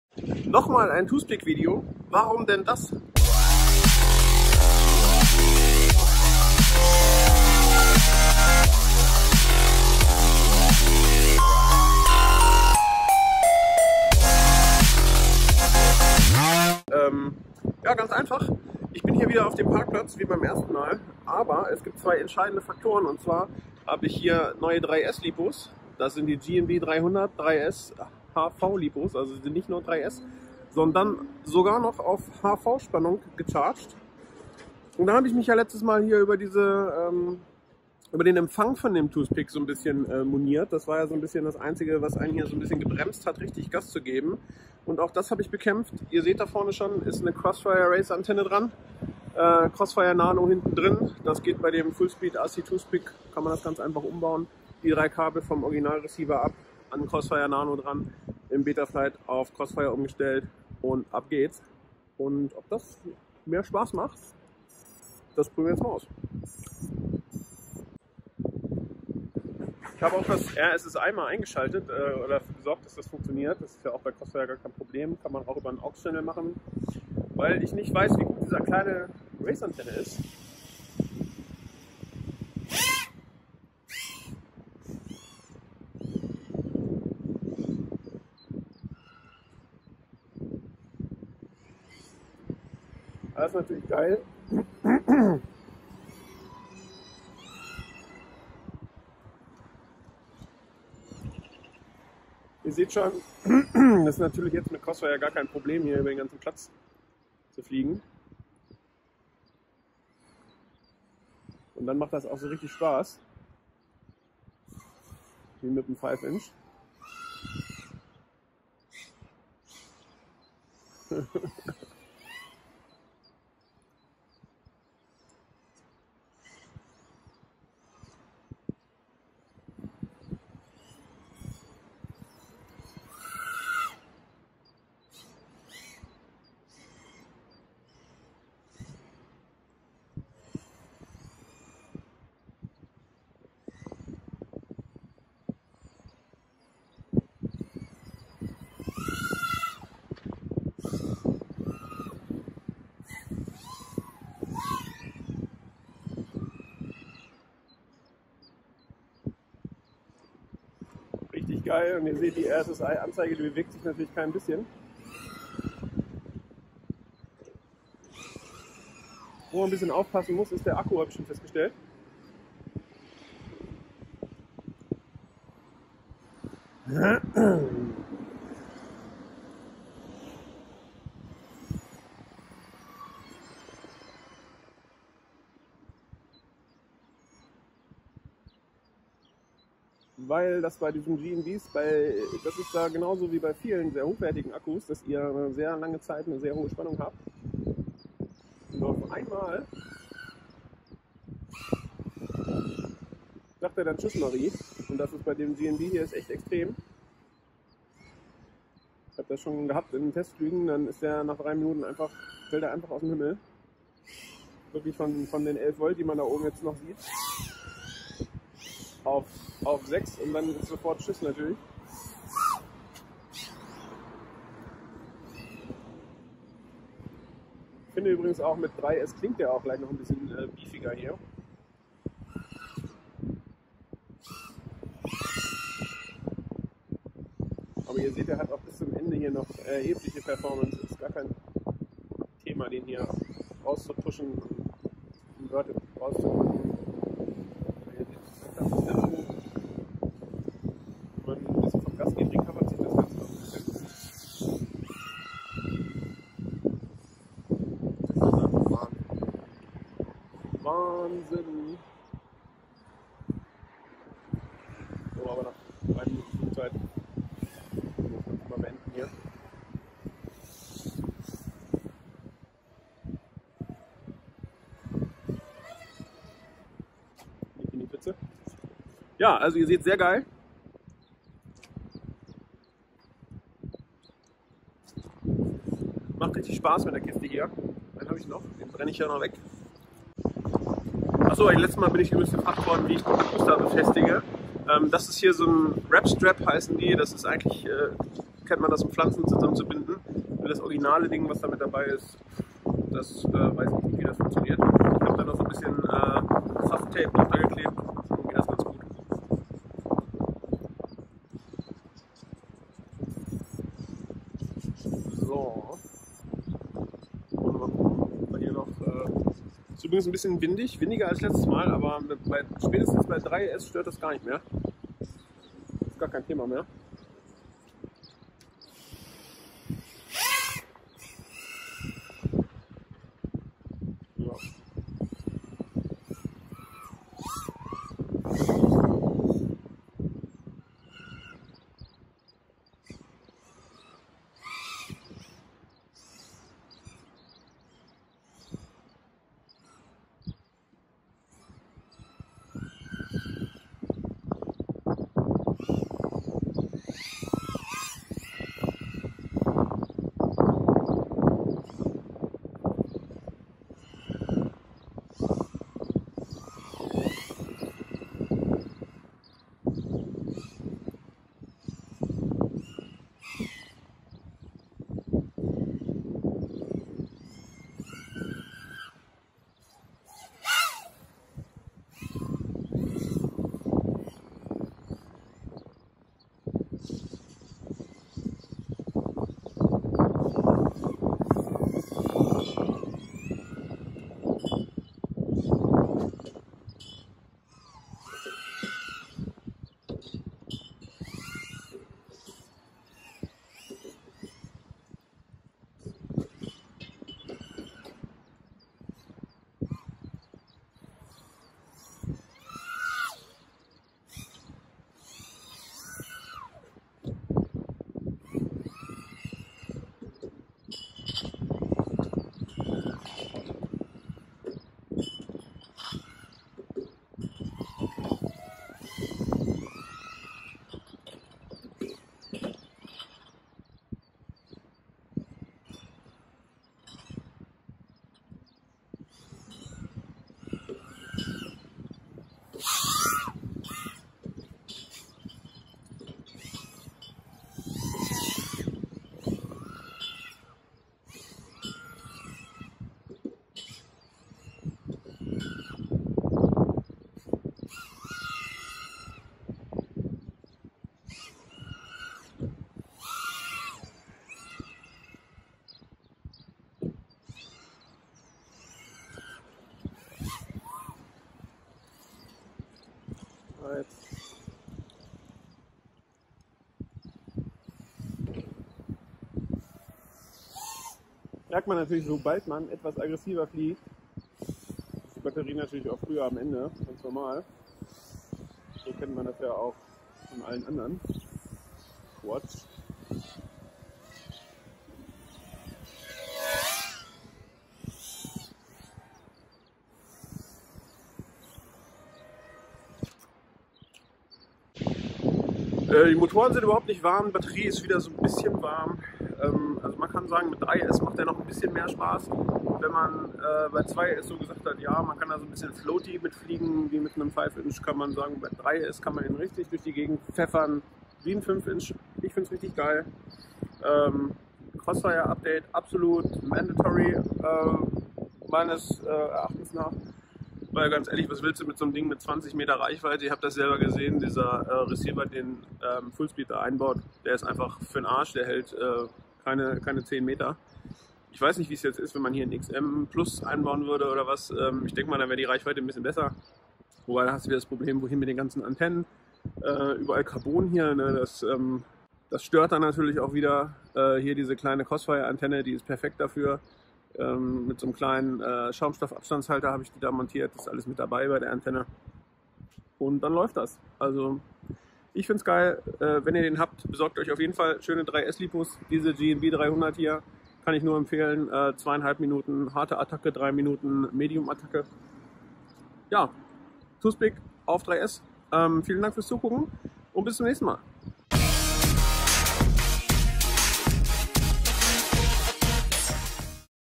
3S und Crossfire im Toothpick?! Aber ja doch! Fullspeed RC Toothpick #2
Hast du Fragen zu den Teilen oder unserer Ausrüstung generell? Bitte lies diese sehr detaillierte Beschreibung.

Produkte die ich in dem Video erwähnt habe:

FullspeedRC Toothpick

GNB 300mah 3S HV

Der FC nutzt das "MATEKF411" Target. Ihr müsst dann Yaw direction auf 270 Grad stellen. Zum flashen müsst ihr den Bootloaderknopf drücken, der ist auf der Unterseite des FC.

Sonstige Ausrüstung:

Video Gear:
Canon G7X mkii
Bendy Tripod